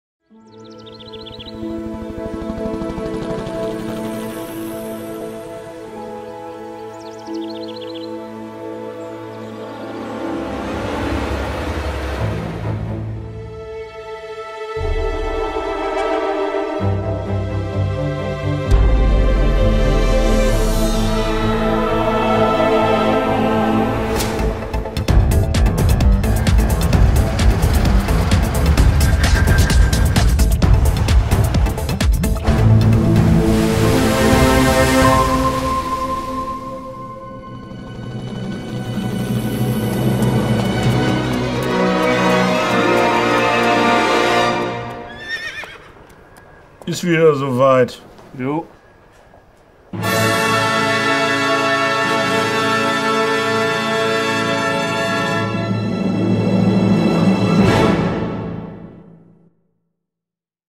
Trailer Kinofest Lünen 2018
Unser Trailer zum Kinofest Lünen 2018, gedreht in Lünen mit Lünern und Waldemar Kobus; Idee und Umsetzung von Axel und Henning Ricke von Lumatik Film, Köln.